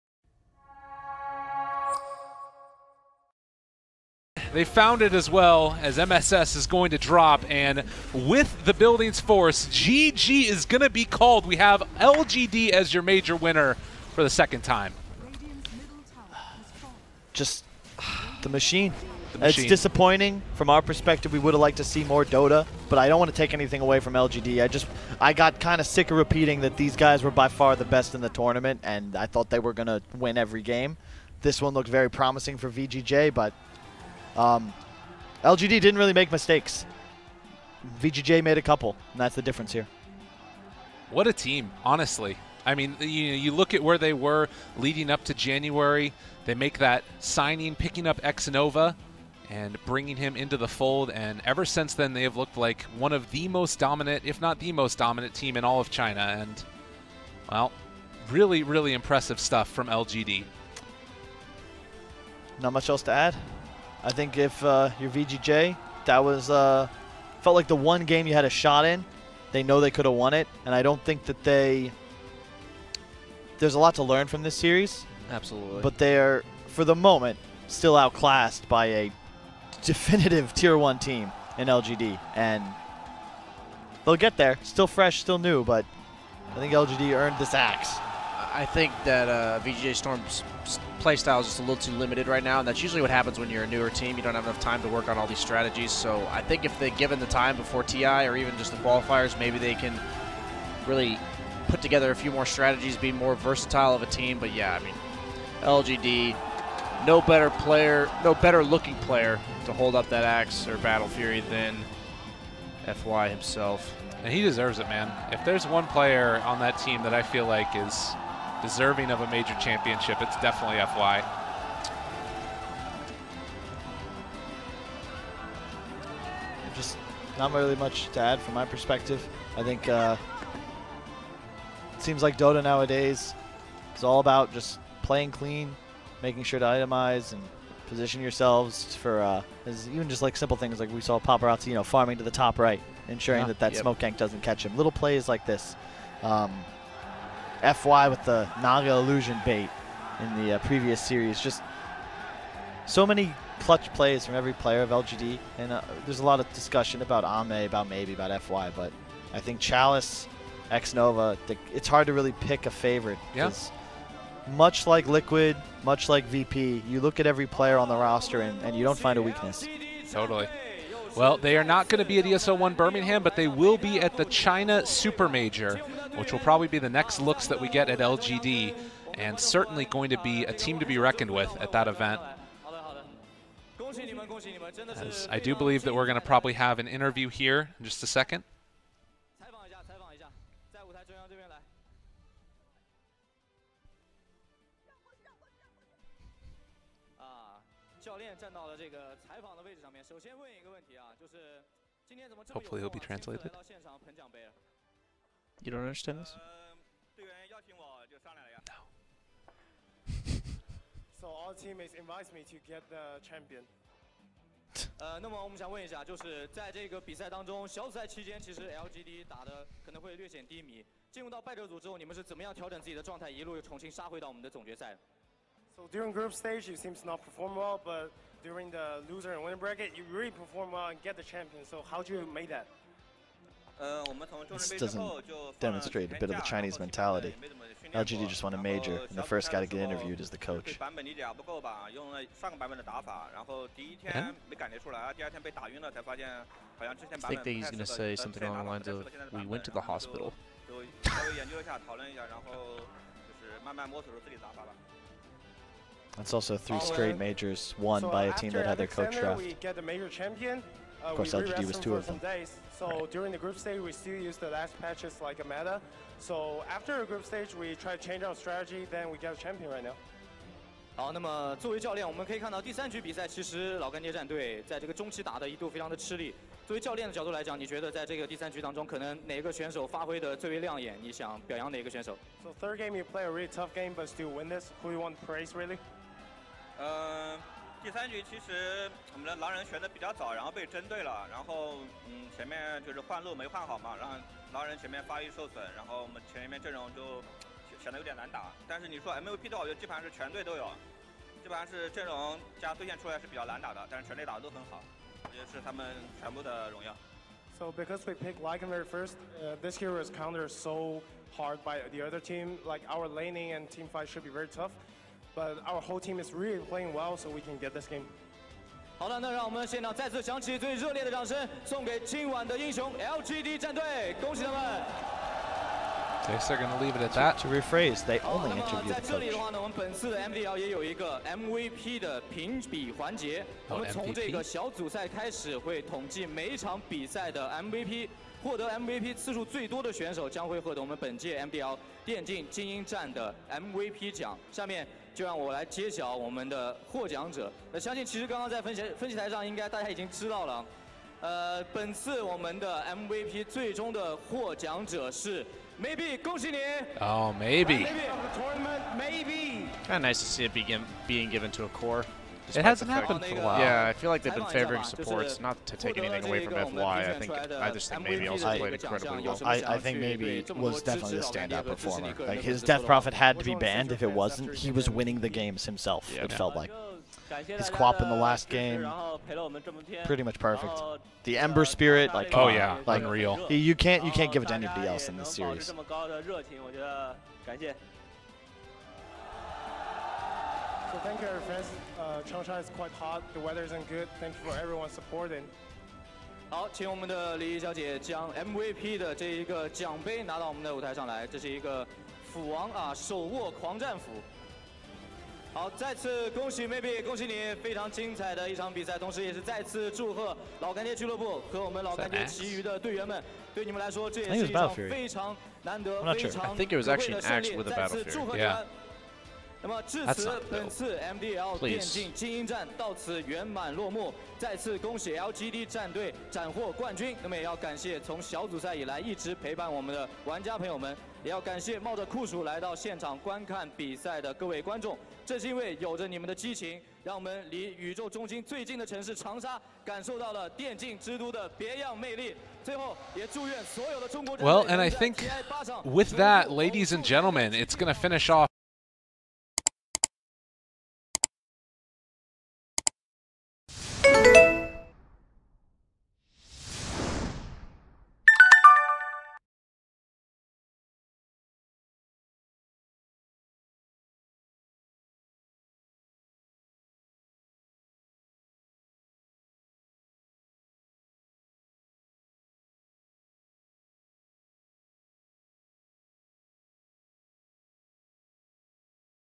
PSG.LGD champion 🏆 MDL Changsha Major champions Grand Final vs VGJ.Storm Winning moment
1. PSG.LGD — $400 000
2. VGJ.Storm — $200 000
3. Vici Gaming — $130 000
4. Newbee — $85 000
5-6. Mineski, Team Secret — $40 000 each
7-8. OG, TNC Pro Team — $25 000 each
9-10. Invictus Gaming, iG Vitality — $17 500 each
11-12. Vega Squadron, Infamous — $10 000 each